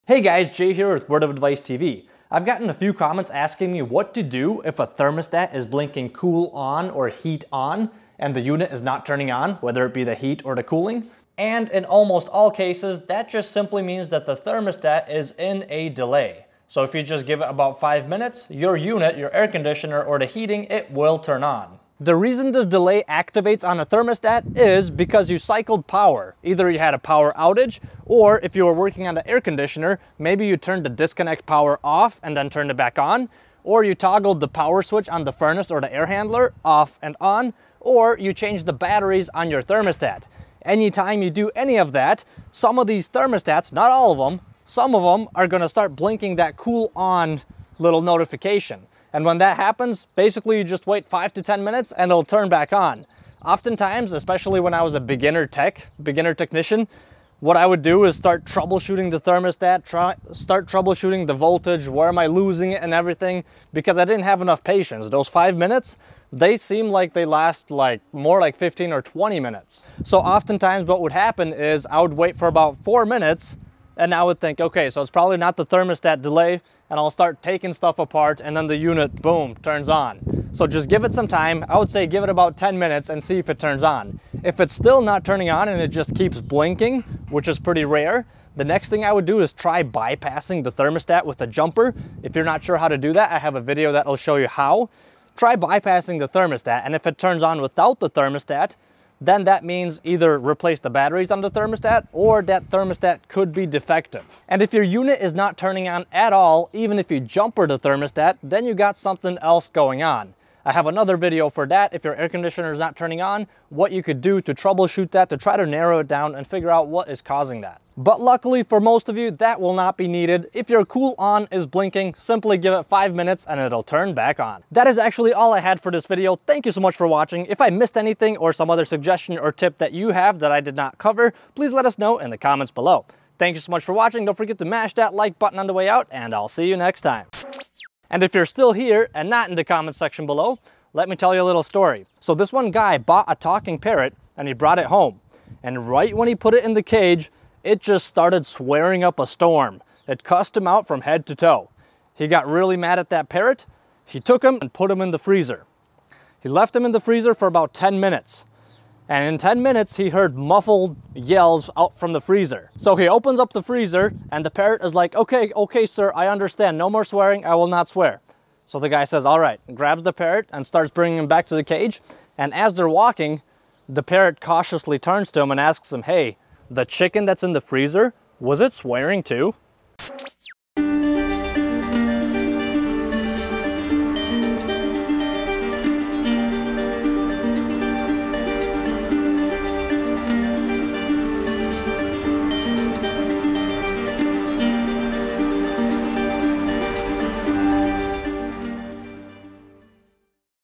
HVAC Thermostat Blinking Cool On But A/C Not Working
Is you house HVAC thermostat blinking "cool on" or "heat on" and does not turn on the furnace or air conditioner? This is most often caused because power was interrupted/cycled even if the interruption was for a very brief moment. Some thermostats like the Honeywell brand have a delay timer which is used to prevent the heating and cooling equipment from coming on too quickly (known as short cycling). Short cycling an air conditioner can damage the compressor which is why some thermostats have a compressor protection feature built in that prevents the compressor in the HVAC unit from coming on immediately after power is reset. The delay is usually 5 minutes long and once the "cool on" or snowflake stop blinking and turn solid, the unit should come back on.